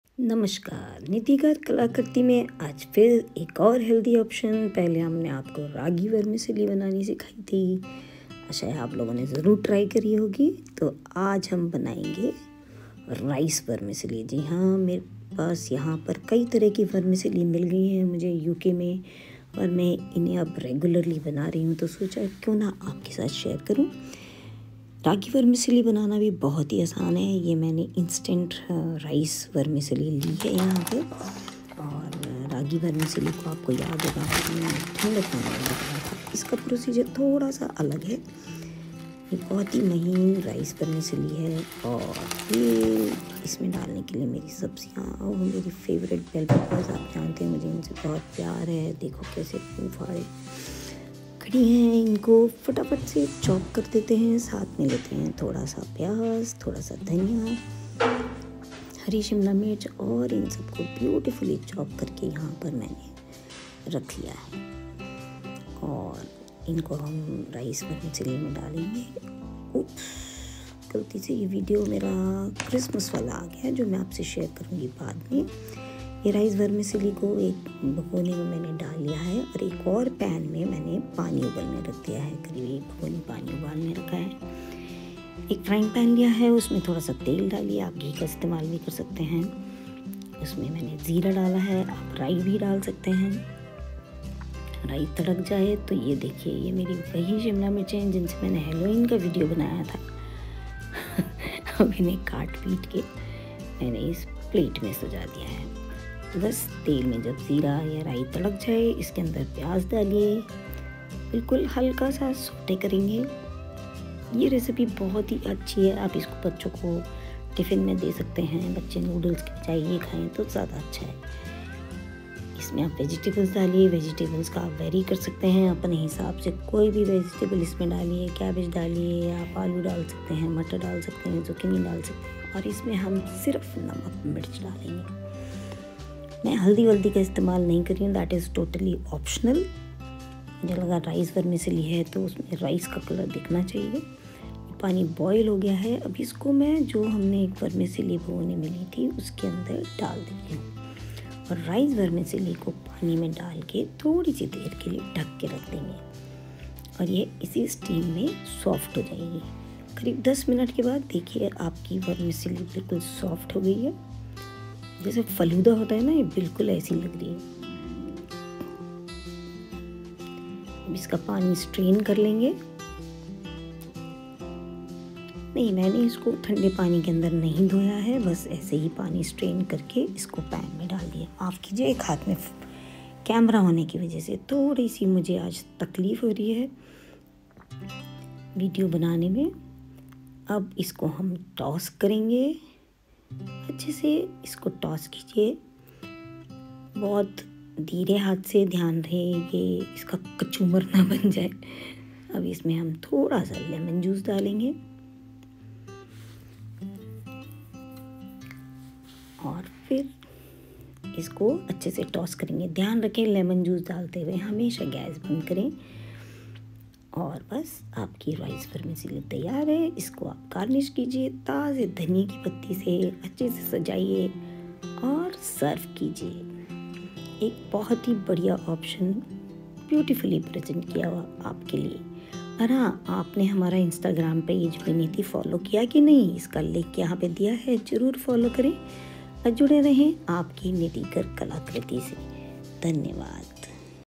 Rice vermicelli
Just with basic ingredients rice vermicelli can be made. You just need to put hot in vermicelli and put it to rest for ten min. Till then sauté vegetables in very little oil. Add salt and pepper. Adding peanuts is optional. Now strain vermicelli and add it to the sautéed vegetables. Toss well. Switch off and add lemon juice. Garnish with coriander leaves  and serve.
It’s a very good option for kid’s lunch. Quick to make .
Love you all ❤️